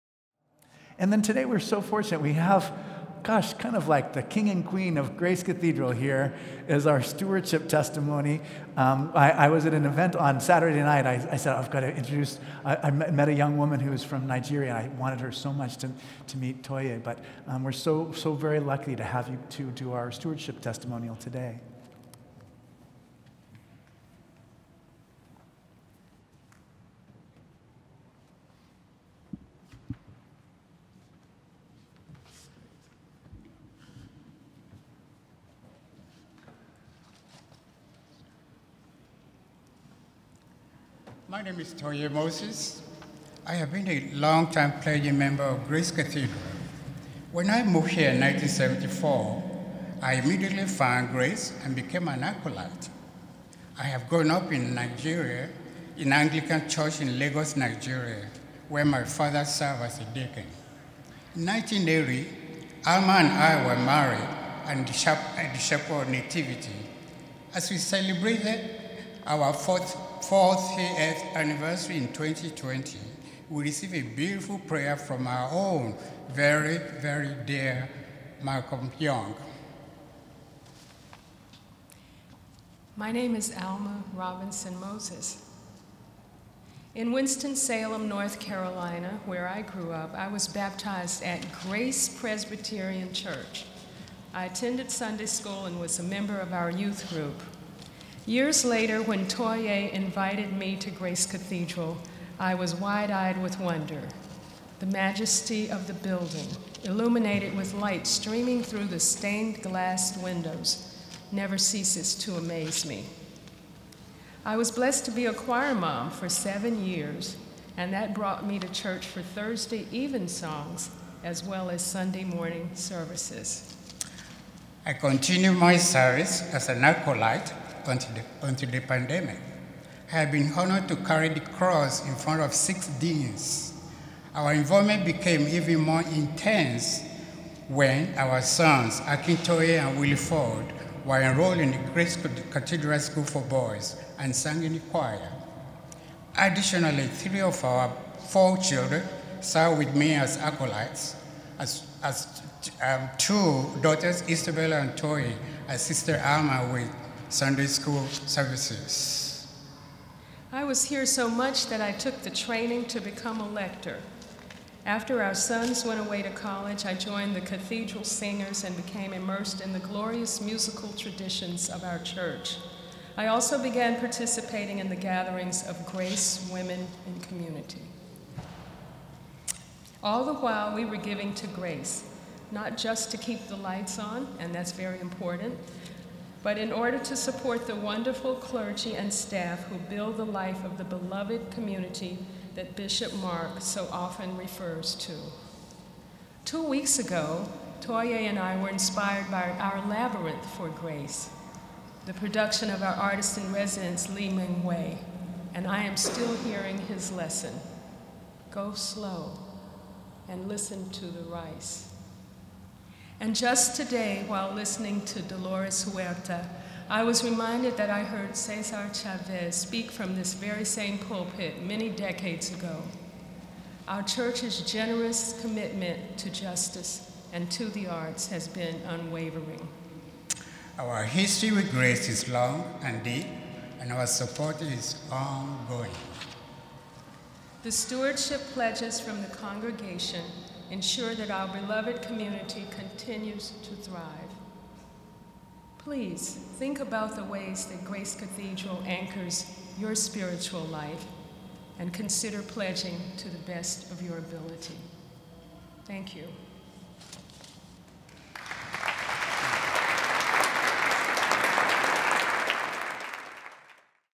Stewardship Stories – Hearts Connected: Alma Robinson and Toye Moses
Hear from Alma Robinson and Toye Moses on why they are 100% Grace at our 10/23/22 Sunday Choral Eucharist.
Fall is stewardship season in the Episcopal Church. It is a time for us to show our appreciation for the love we experience in our caring, inclusive community. Our participation in the stewardship campaign means that we make the ministry and message of Grace possible.

When we pledge, we become a part of something bigger than each of us individually.